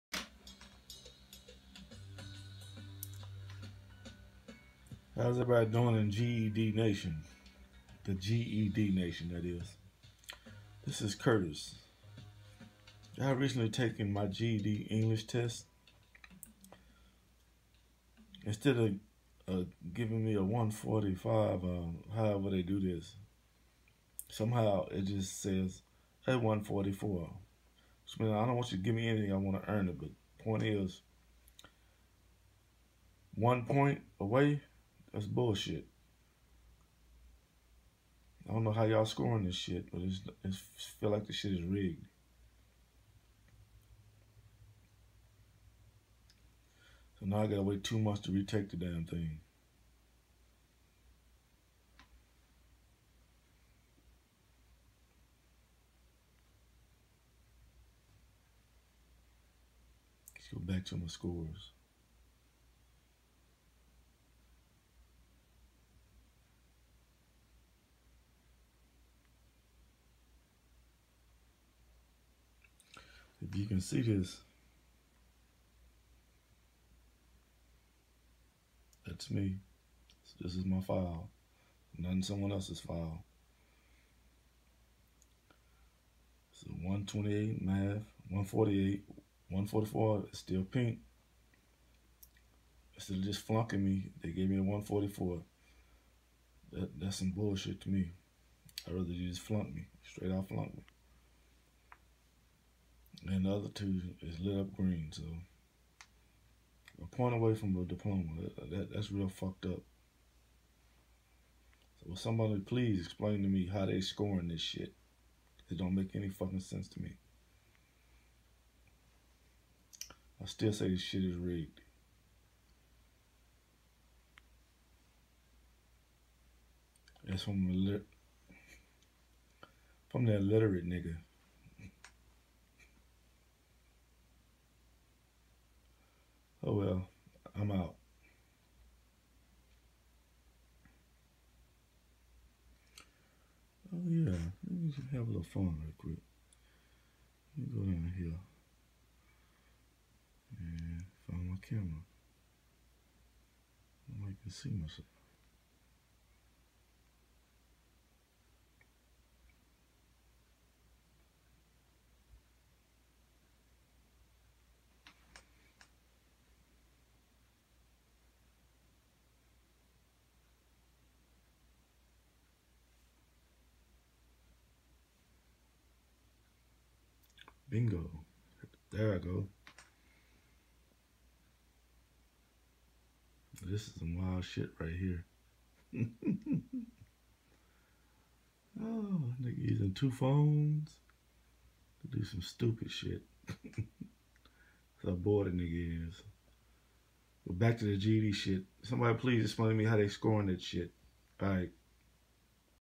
My g.e.d 2017
How the ged is screwing Us on this test